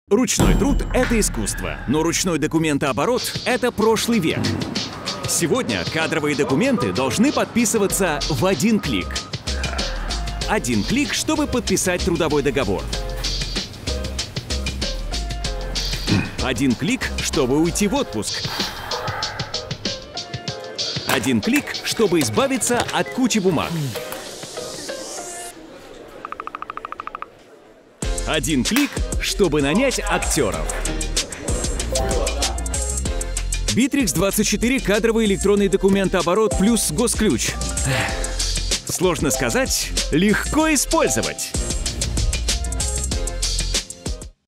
Битрикс24 КЭДО + Госключ. Удобно, быстро, без доплат
Для подписания документов больше не нужно идти в офис, отправлять курьера или оплачивать электронные ключи.

Поставить подпись можно онлайн в Битрикс24. И там же работать с задачами и чатиться — всё внутри одного сервиса😎